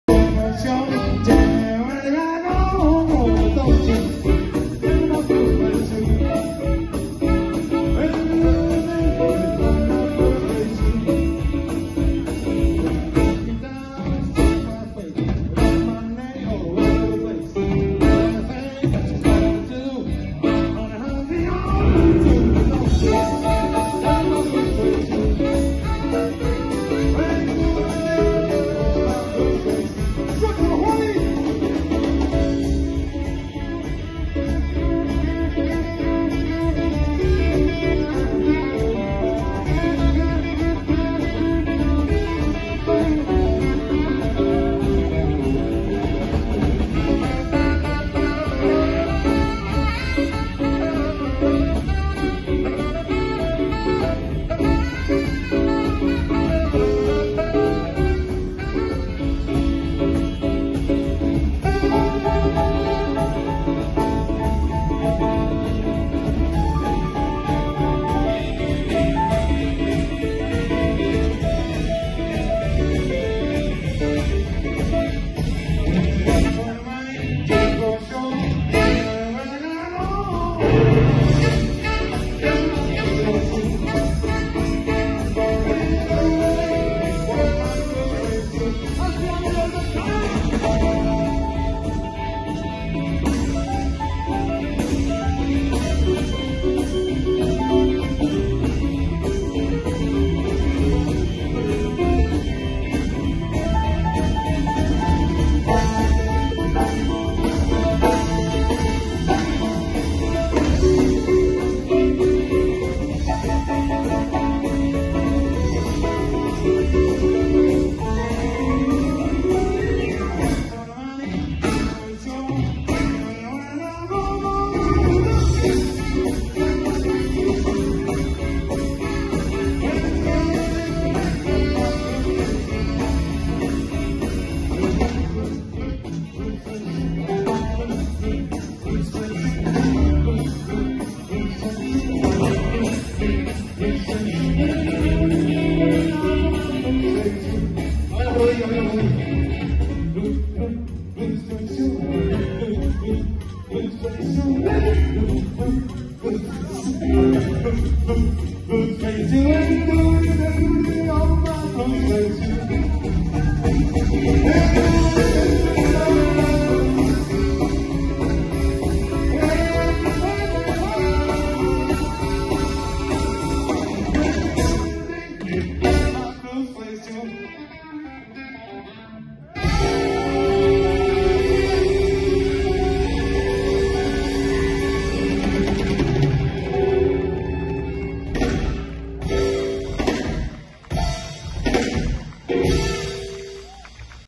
- Taknara Band
Taknara - CENTRO CULTURAL LA CHARCA. 25 ANIVERSARIO ASOCIACIÓN DE VECINOS LA MOLINA DE LA CHARCA DÍA 10 DE JUNIO. 2023. PUERTO DEL ROSARIO.”